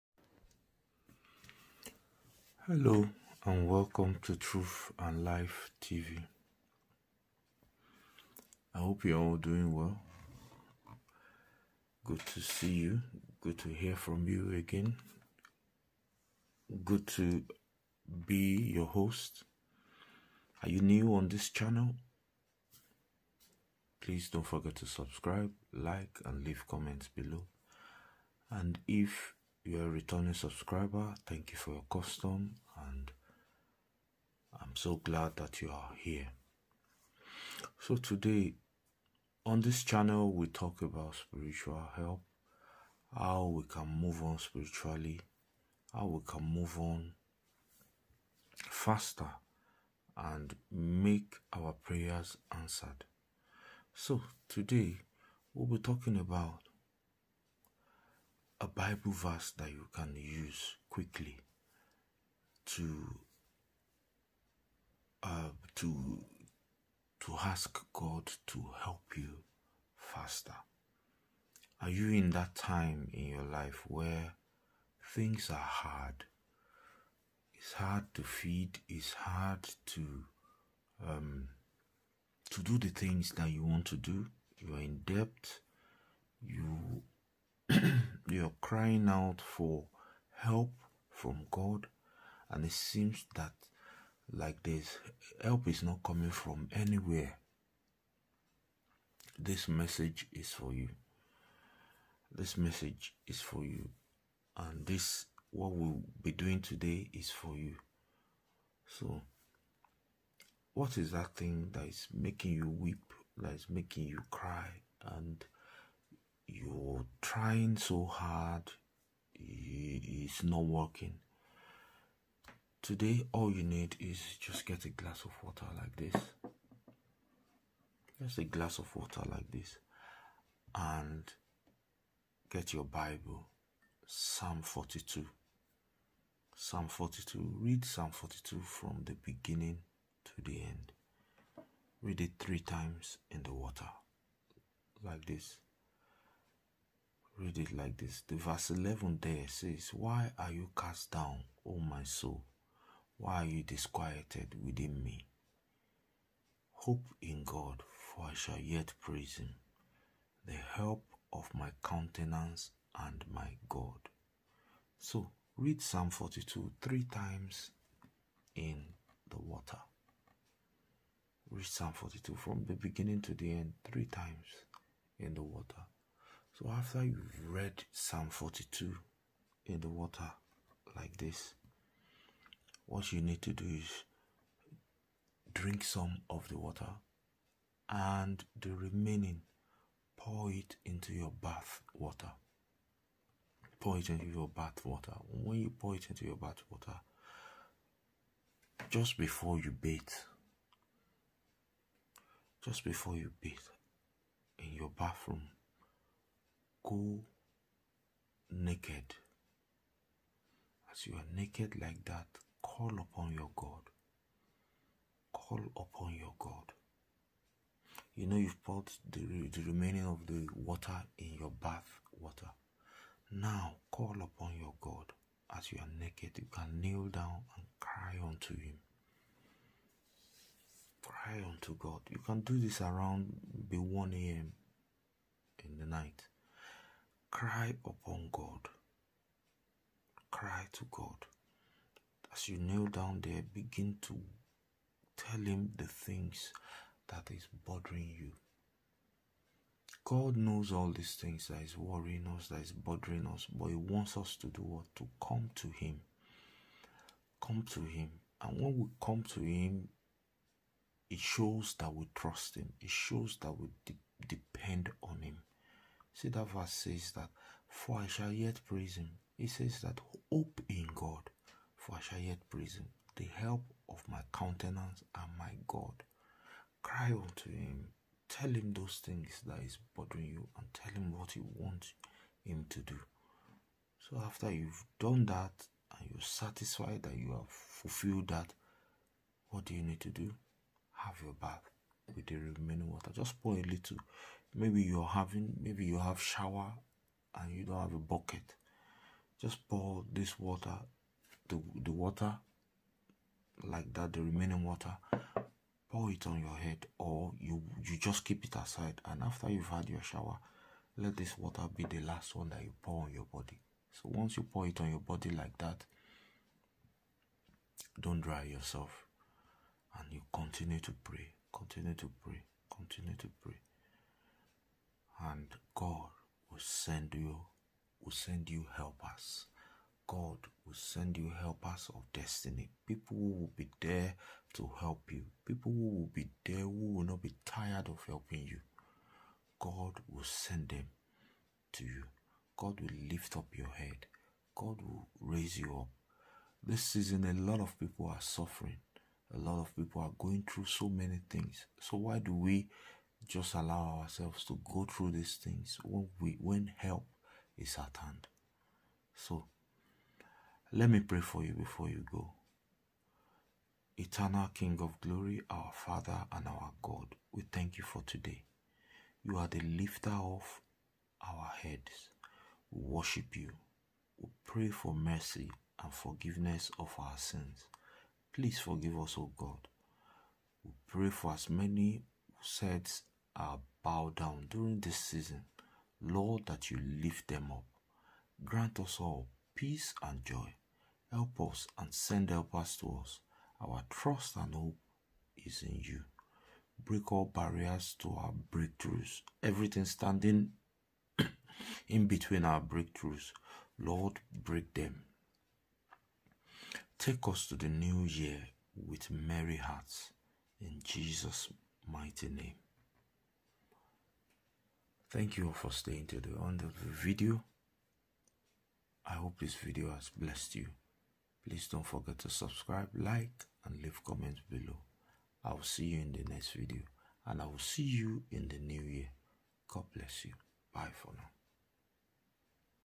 Pray With Psalms 42 This Way For Fast Answer
Pray With Psalms 42 This Way For Fast Answer when things are not working as it should

if you have a need for prayer? then let's join our faith together send an email
i do not give phone numbers or whatsapp pls beware of scammers.